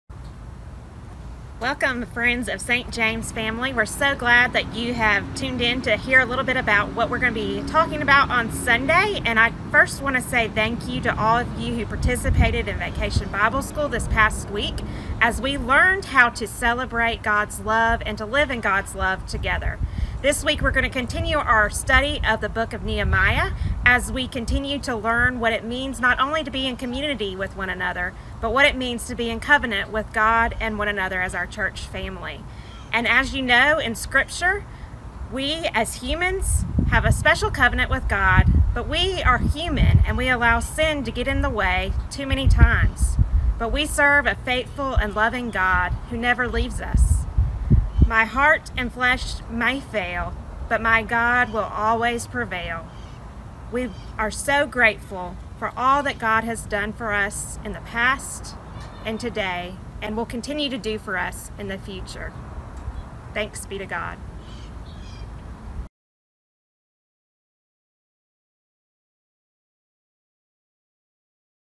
Preview to Sunday's sermon
Pastor Martha Timmons continues are Daring to Dream sermon series.  She will share with us what it means to be in covenant with God and one another as our church family.